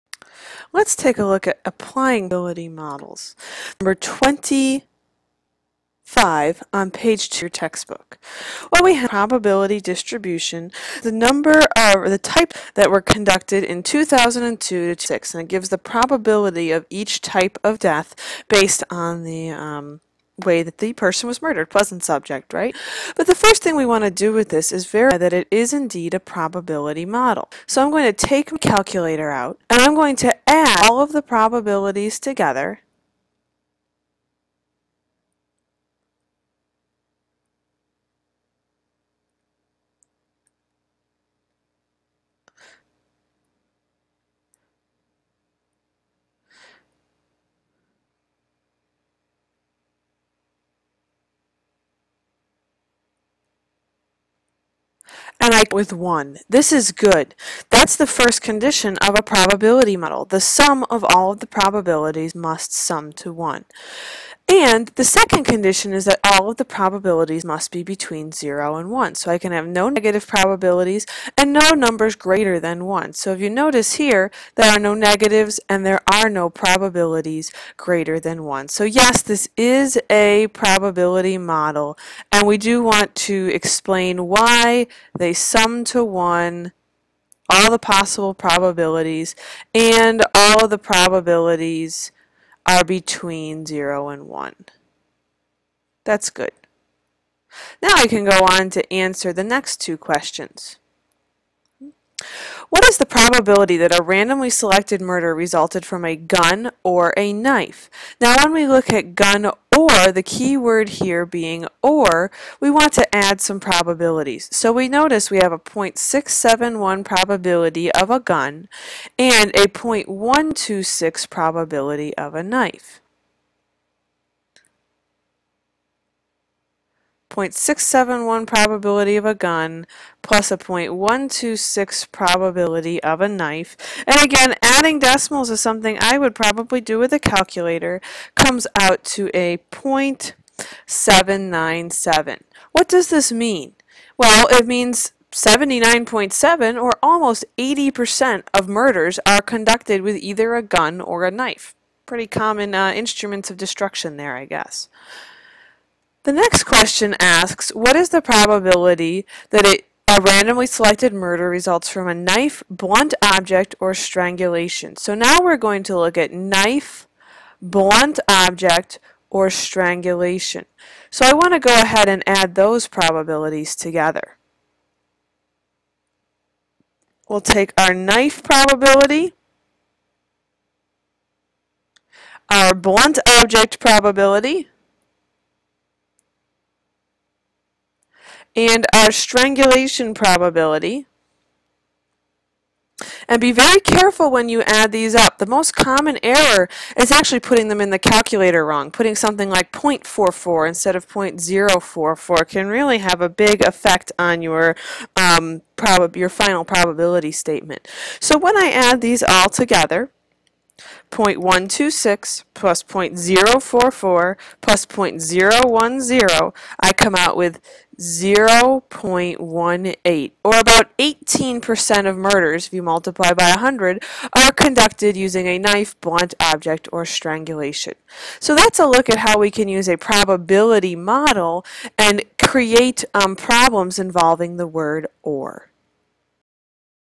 Probabiltity model and OR problems
Using a probaiblity model to answer questions involving OR